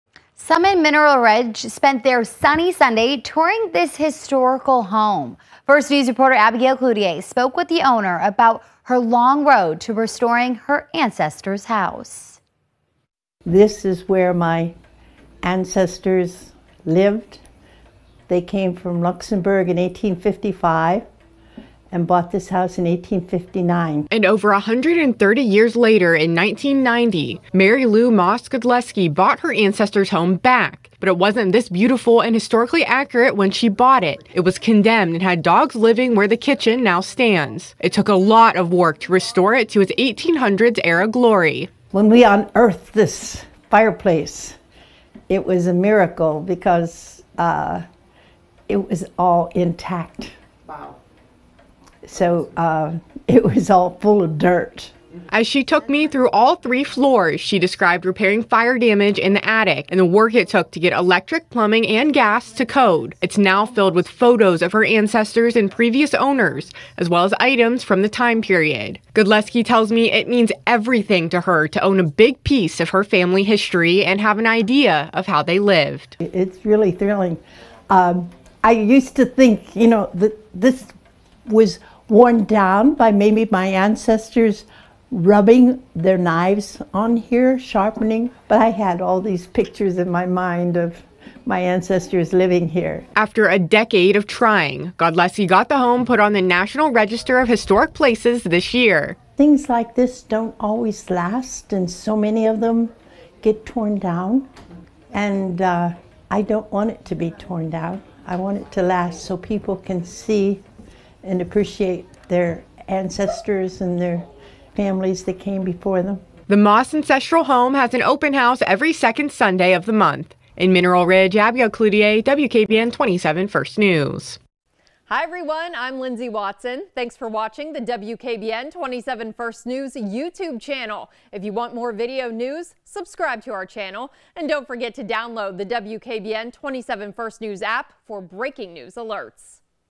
Local woman restores ancestors’ historical home to original glory
Mary Lou Moss Godleski repairs Moss Ancestral Home in Mineral Ridge, Ohio